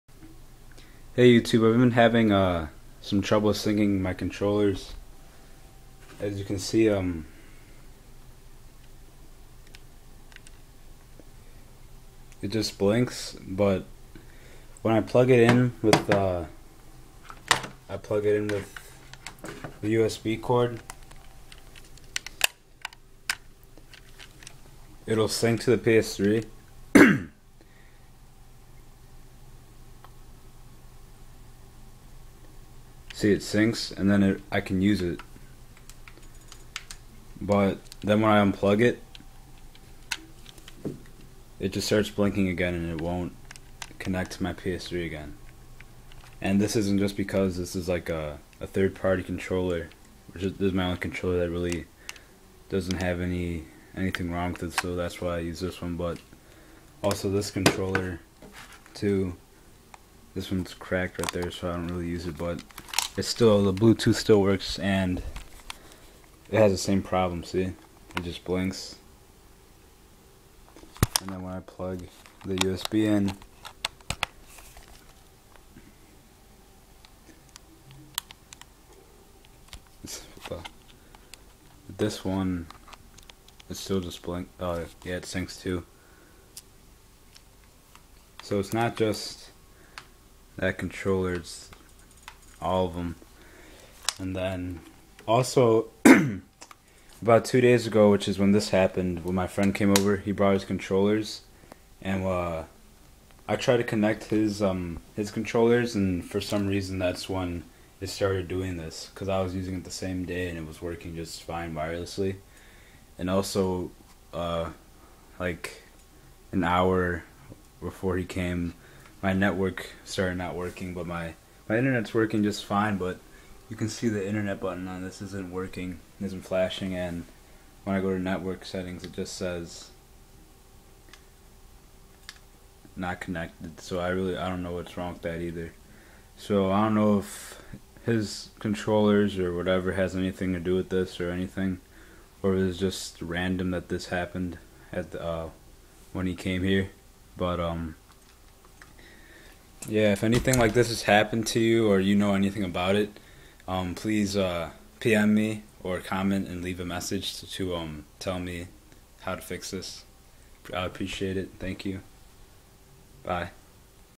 PS3 controller syncing and network problems
My ps3 controllers do not work wirelessly and my network is not connected even though it's working just fine